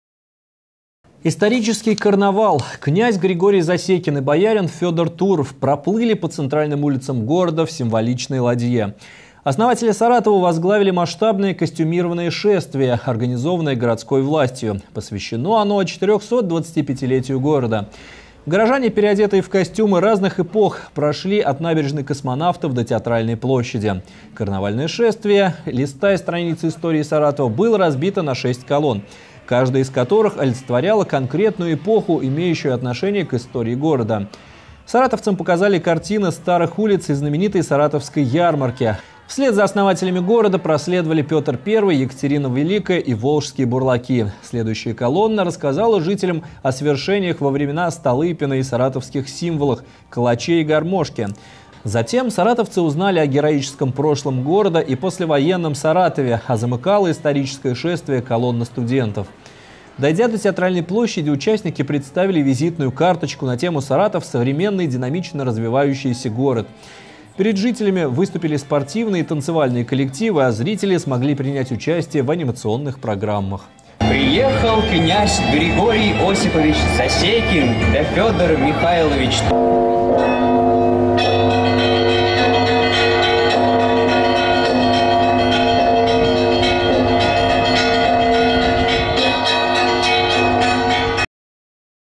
Исторический карнавал: князь Засекин и боярин Туров "проплыли" в ладье
Князь Григорий Засекин и боярин Федор Туров "проплыли"
по центральным улицам города в символичной ладье. Основатели Саратова возглавили масштабное костюмированное шествие, организованное городской властью. Материал вышел в эфир 14 сентября 2015 года.